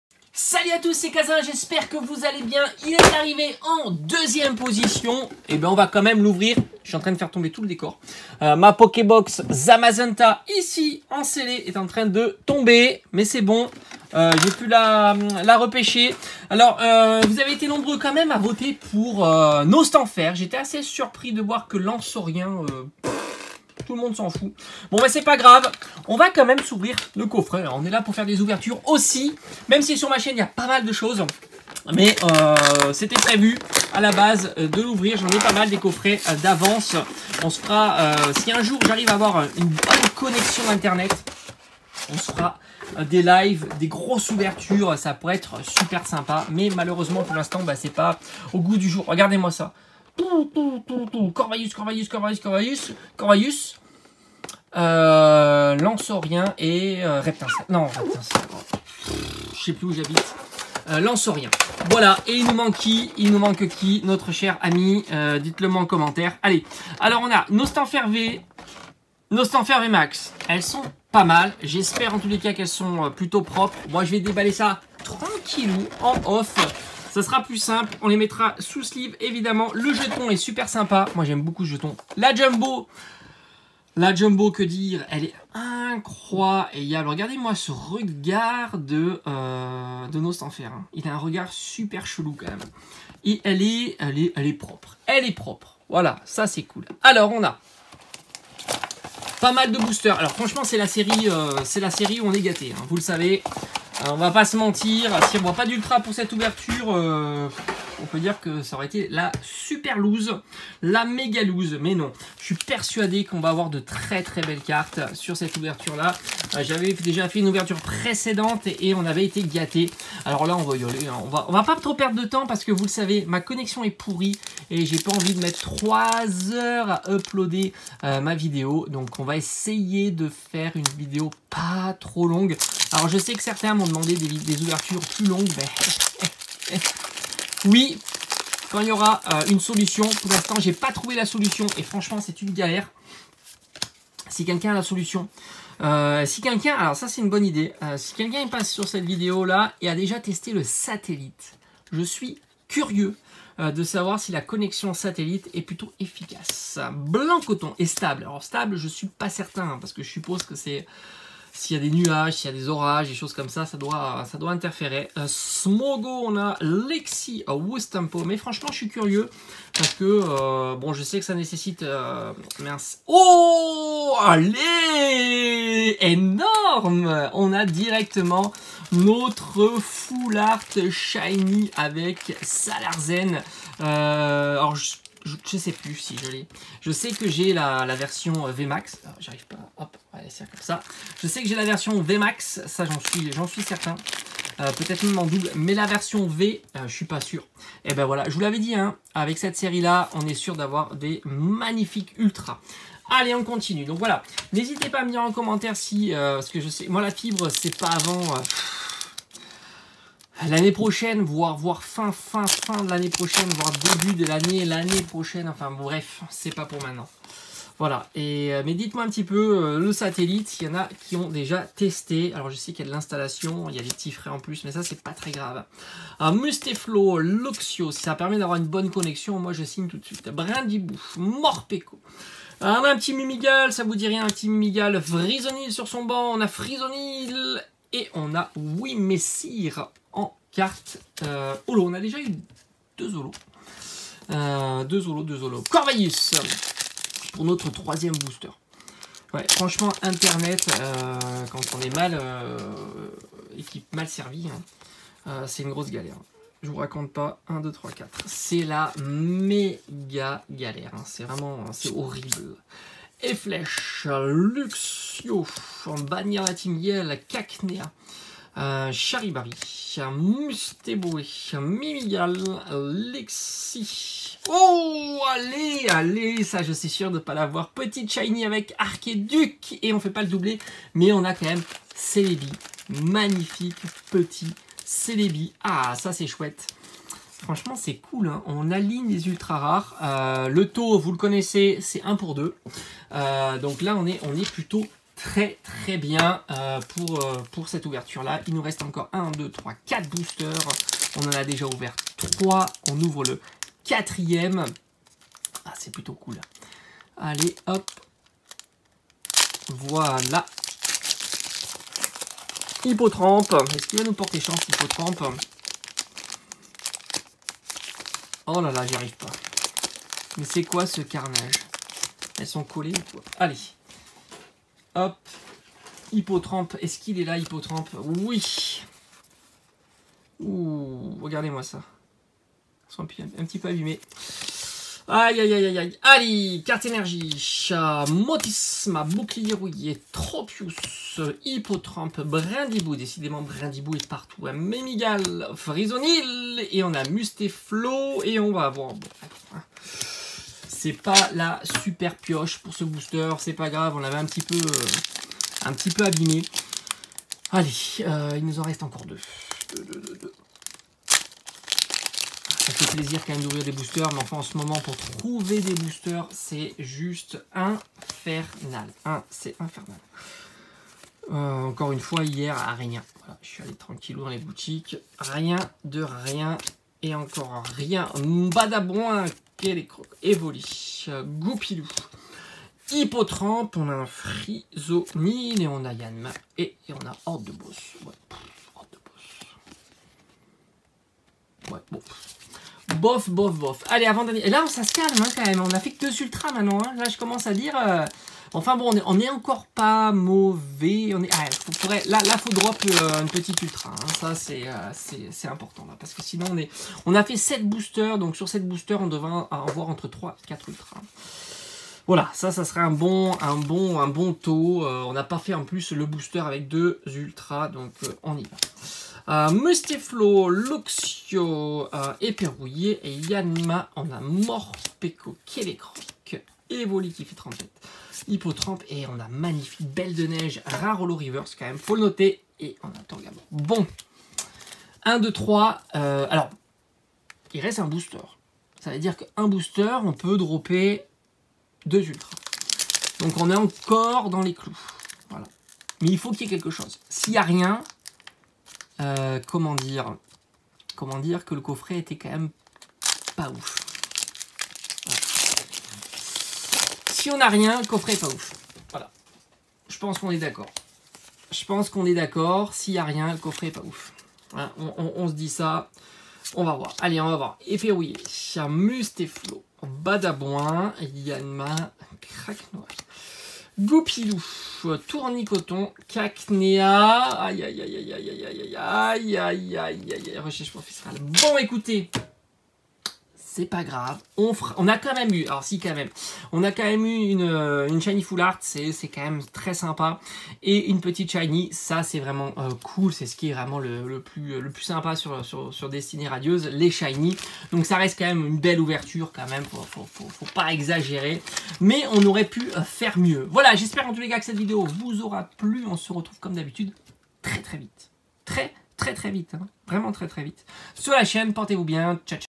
On Se l'Ouvre Quand meme Ce Coffret Destinées Radieuses
Quel Plaisir d'ouvrir du Destinées Radieuses  Coffret Pokémon Nostenfer